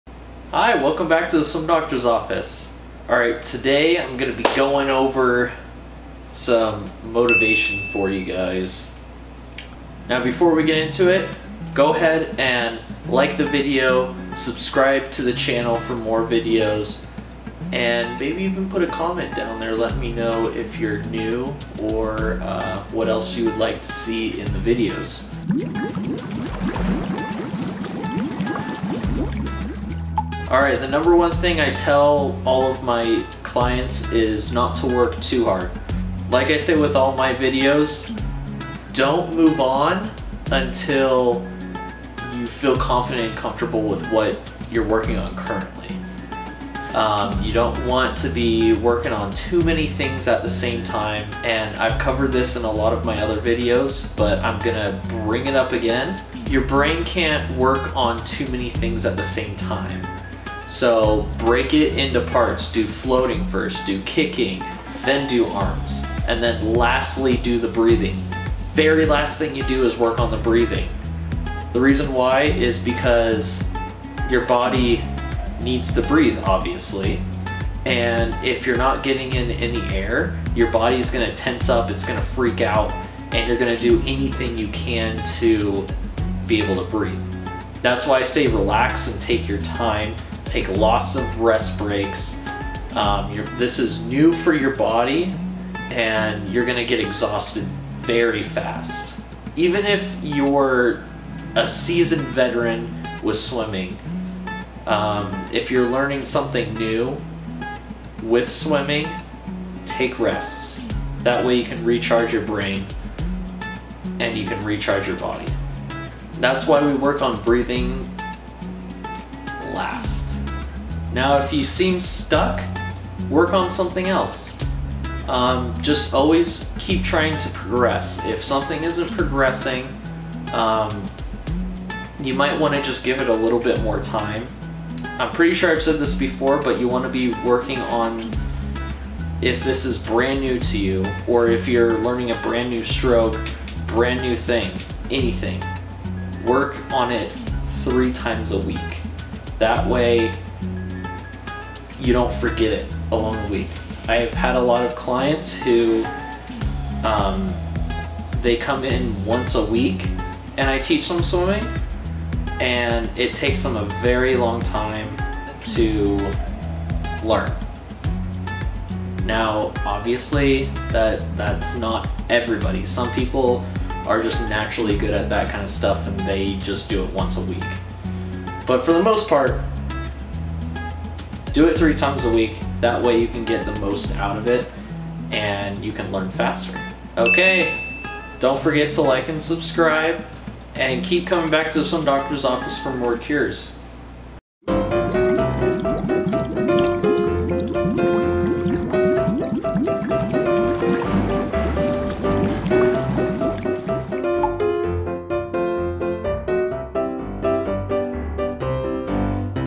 The Best MOTIVATIONAL SPEECH For New SWIMMERS!! +Tips
In the video the Swim Dr will give you some motivation to help you keep going even when you don't see as much progress as you would like. I also have some tips for you and some experiences from when I taught others how to swim. I want to thank you all for helping me get to my goal of 1000 subscriber!